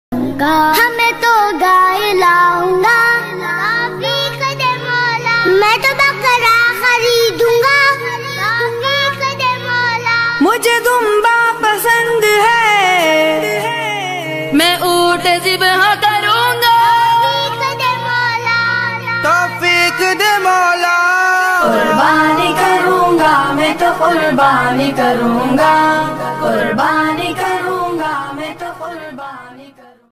Stop cruelty on innocent animals in the name of animals
On festival human beings Killing animal for happiness of God. But all that only thinking of narrow minded people